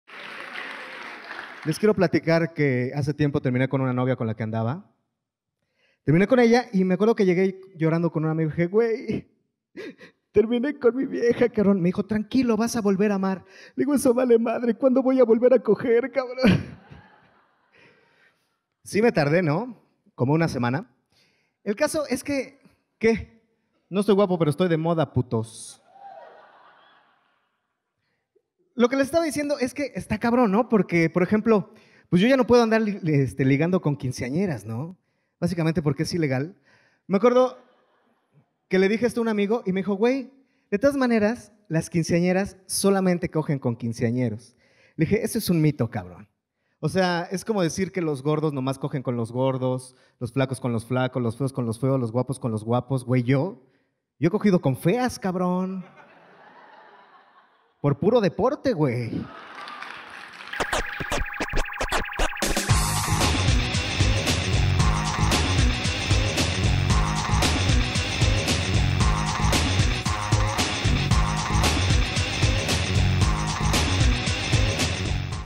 Alex Salazar - Estando solo o en pareja - Mujeres
En este show, el comediante de Ixtapalapa, plantea su monólogo desde su experiencia luego que su novia lo dejara y lo que significa estar solo para tener sexo.